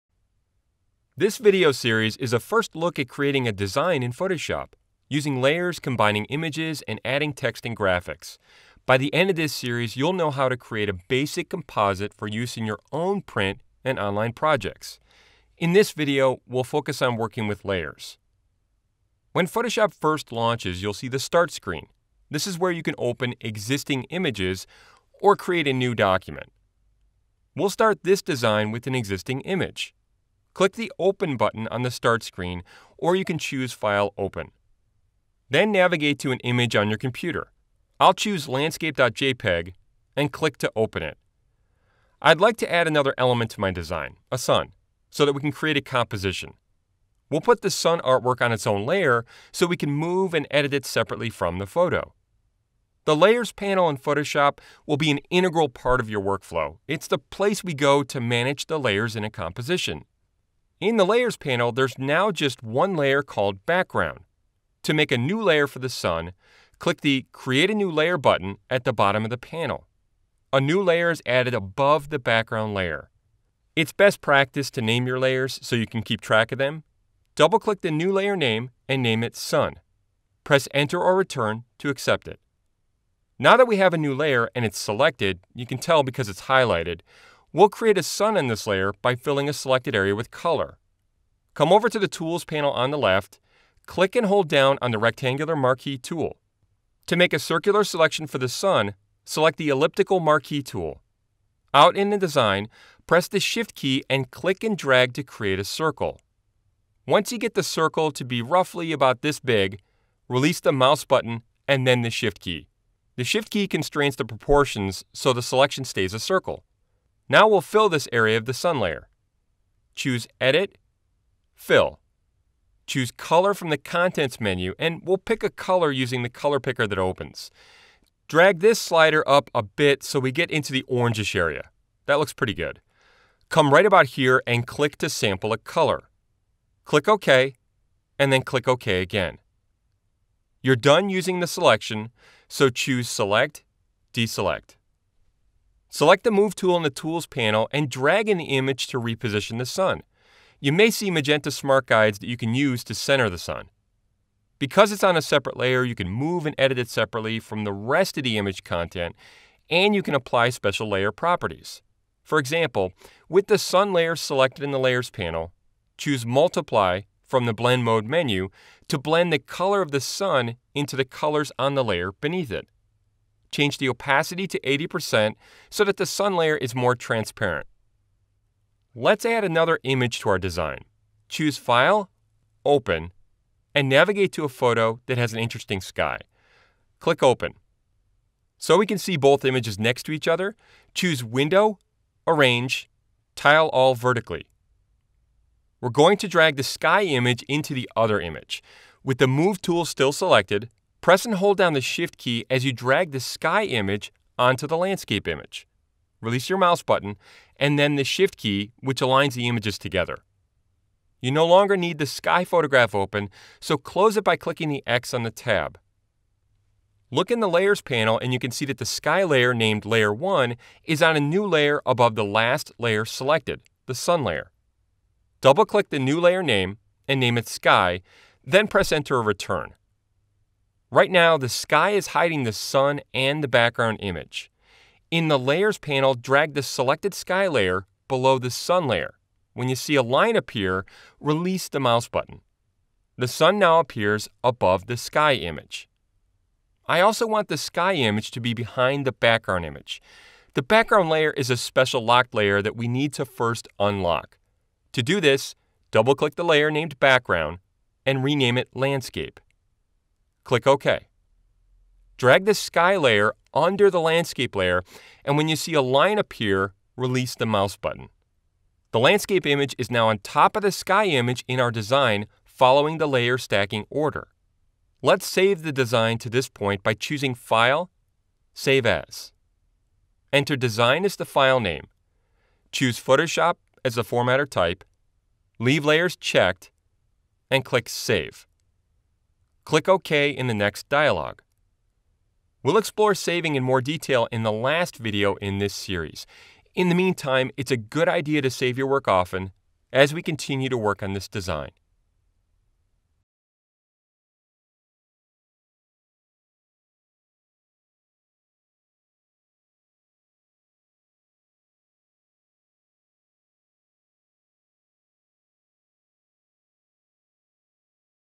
How to Use Layers: Photoshop | Adobe Creative Cloud
Learn how to use layers in this photoshop tutorial.

Learn how to take great photos and make them even better. Preview world-class photography classes from CreativeLive